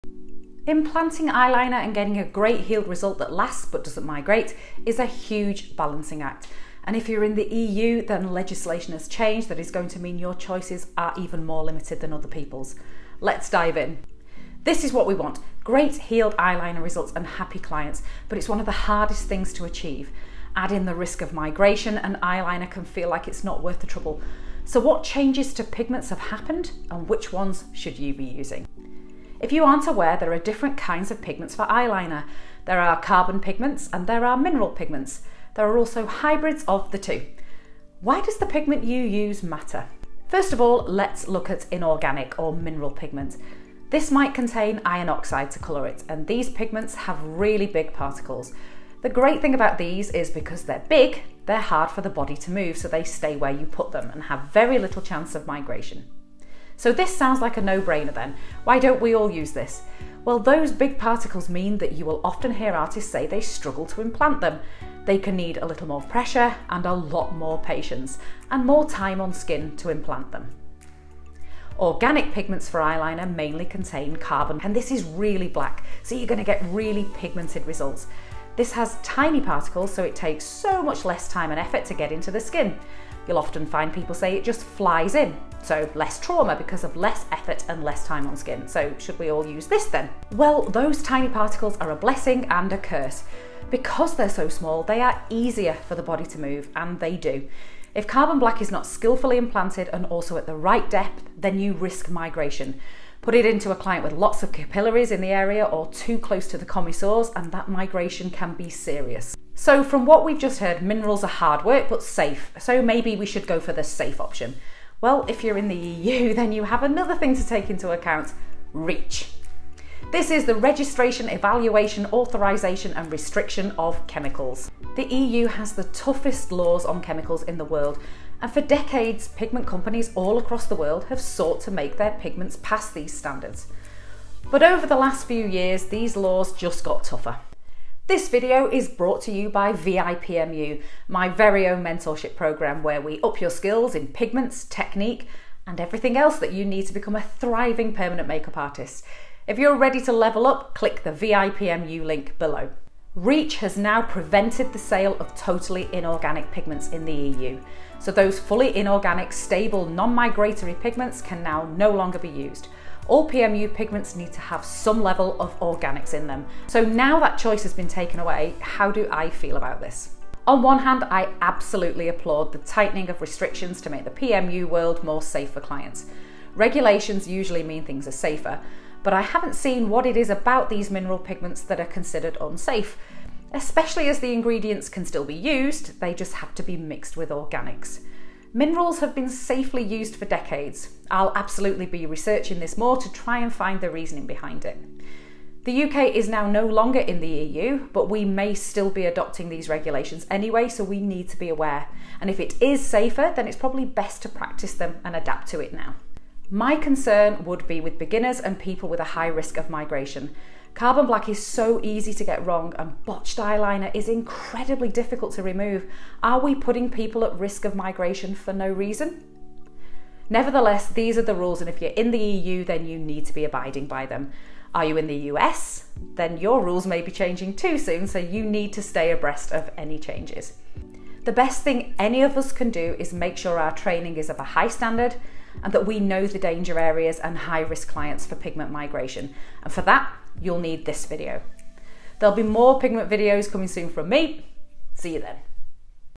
Choose PMU Eyeliner Pigment- Step By Step
Deciding what eyeliner pigment to choose for permanent eyeliner tattooing can be difficult. Choosing between carbon pigments, mineral pigments, organic and inorganic, and also hybrids is tough. Eyeliner migration can be a serious risk so it's wise to have pigmentys for high risk clients, but with new REACH laws in the EU, this is going to affect the production and availibilty of permanent make up pigments in some areas, but also across the world as most pigment production aims to be as stringent as possible.

You should be considering all these things when choosing your pigment and being as knowledgable as you can on technique.

LEARN PMU IN 4 MINUTES A WEEK